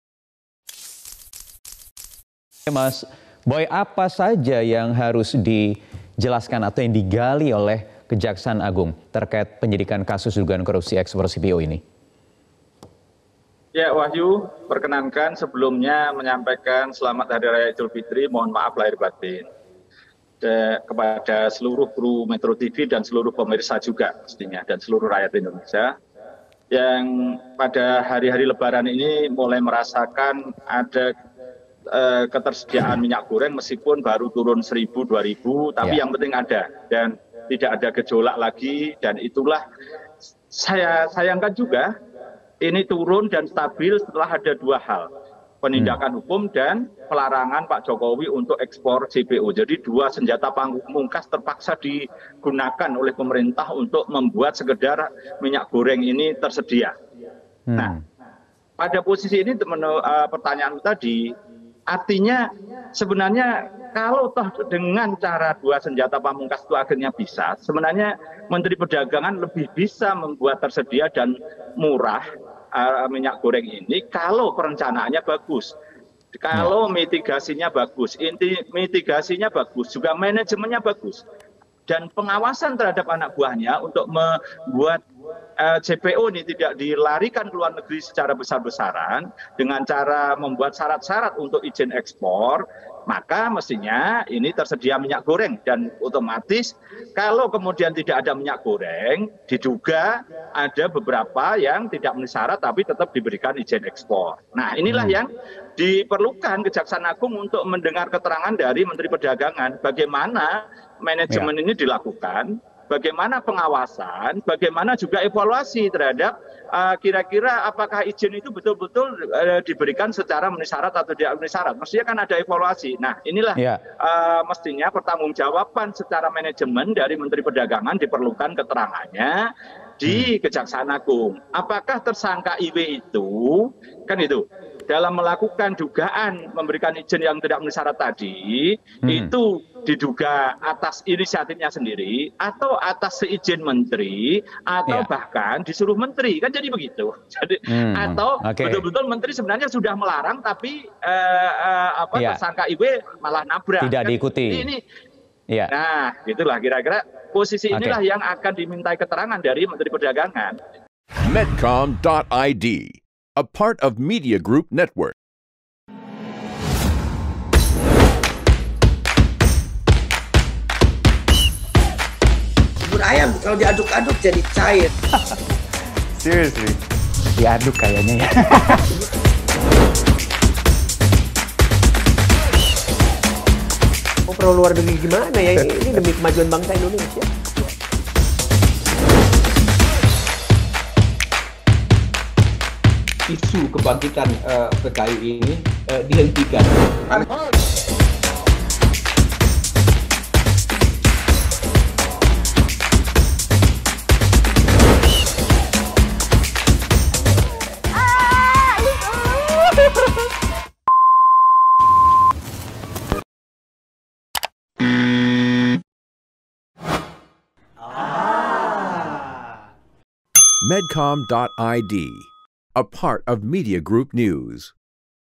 Bongkar Mafia Migor, Mendag Diminta Kooperatif
Jaksa Agung Muda Tindak Pidana Khusus (Jampidsus) Kejaksaan Agung akan memanggil Menteri Perdagangan (Mendag) Muhammad Lutfi usai lebaran 2022.

Menanggapi hal ini, Koordinator Masyarakat AntiKorupsi Indonesia (MAKI), Boyamin Saiman, menyebutkan kejagung harus minta penjelasan mendag terkait manajemen yang dilakukan. Sebab, menurutnya, secara manajemen mendag seharusnya tahu tindakan anak buahnya.